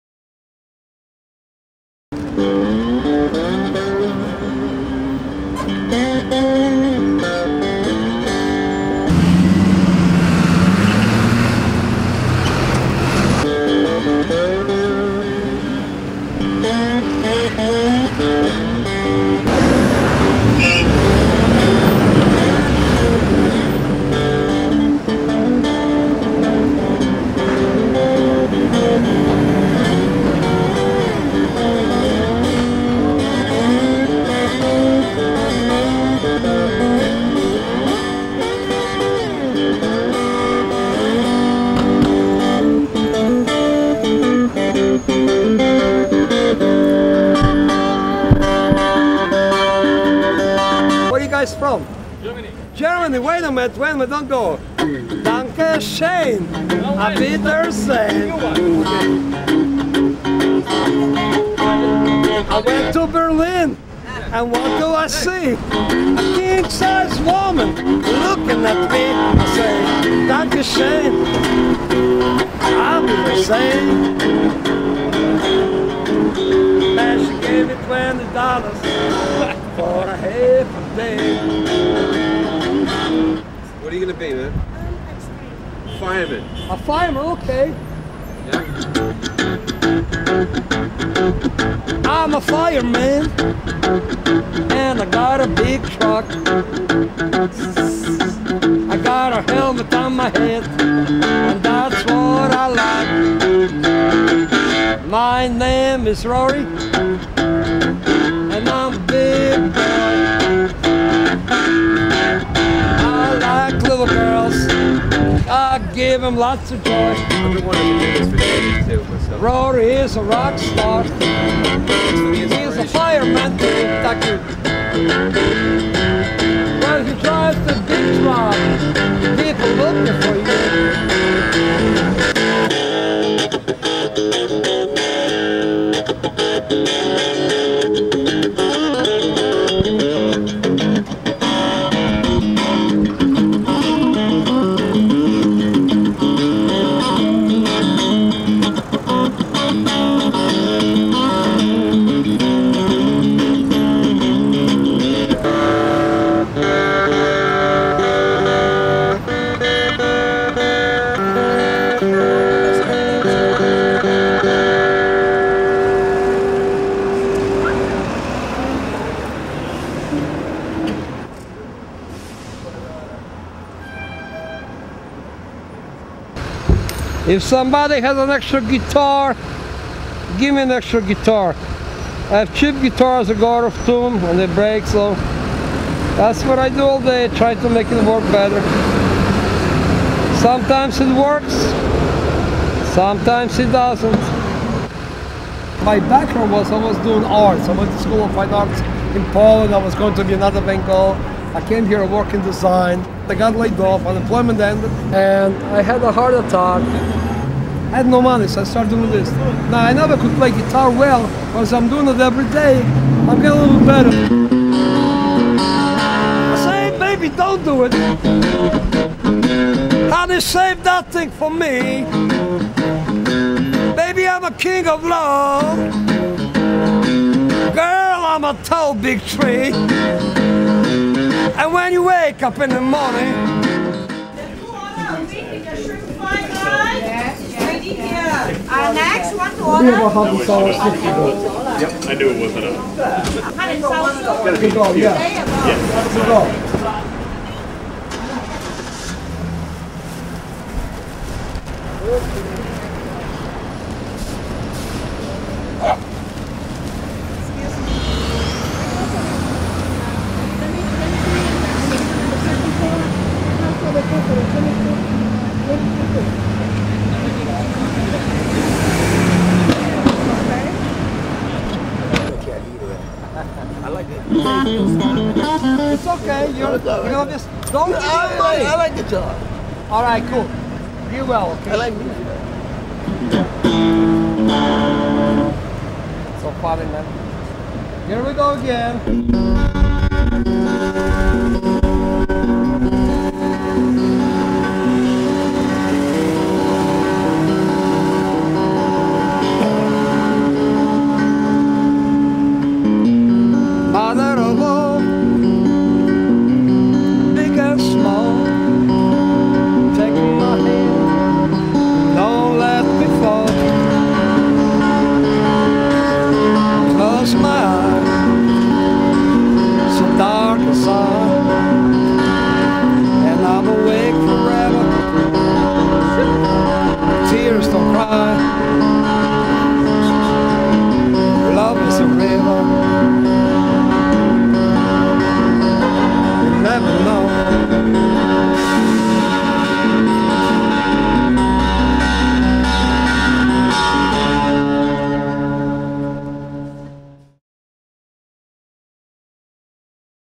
Everyday Blues
Street musician, Vladimir Laksin, plays the blues every day in Chelsea NYC